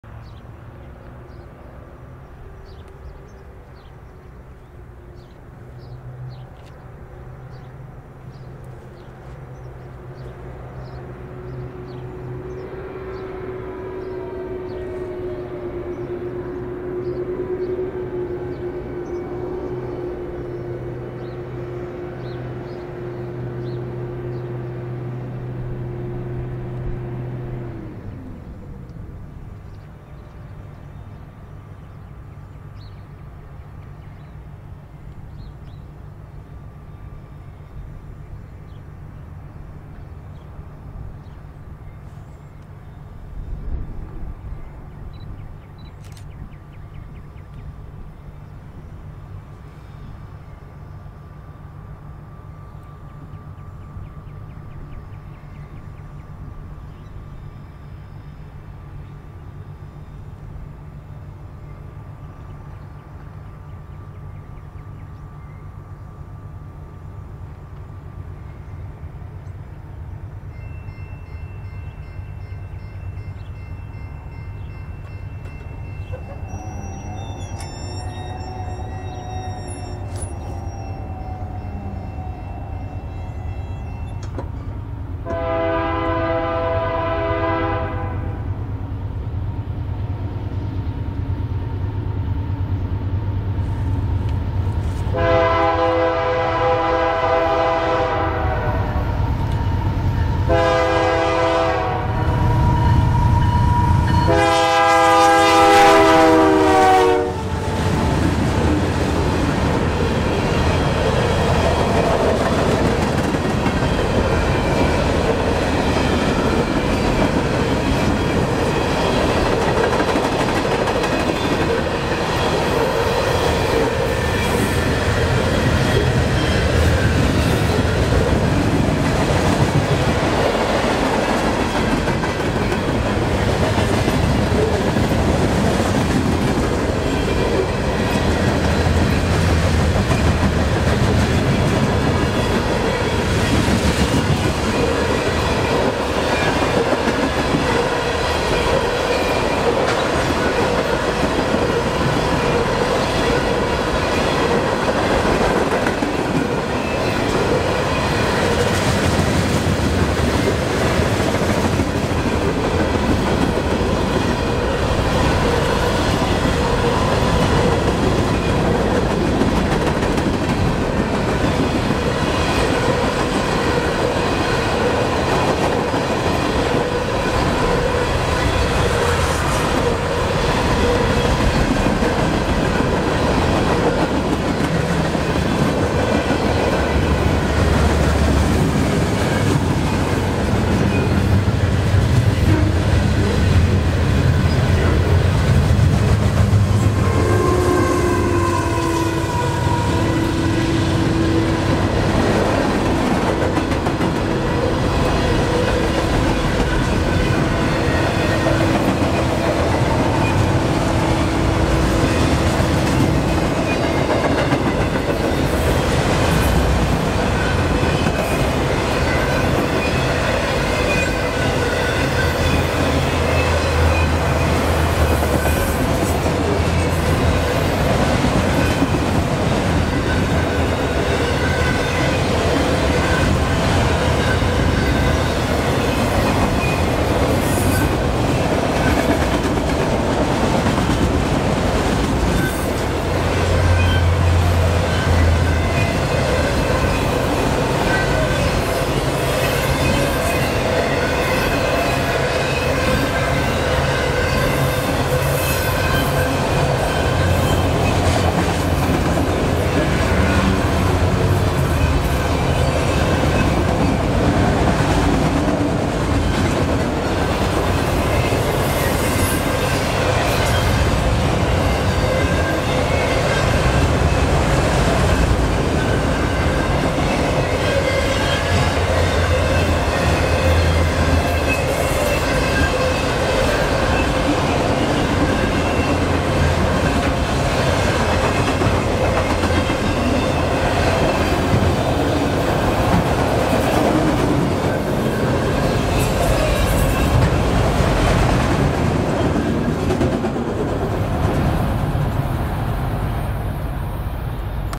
CP 8030 West in Rolling Prairie, IN 3/15/22
WB Manifest, Prairie St., Rolling Prairie, IN, NS Chicago Line
CP 8030 = AC4400CWM - CP w/ Beaver - Narrow Font K3L
CP 8016 = AC4400CWM - CP w/ Beaver (Mid DPU)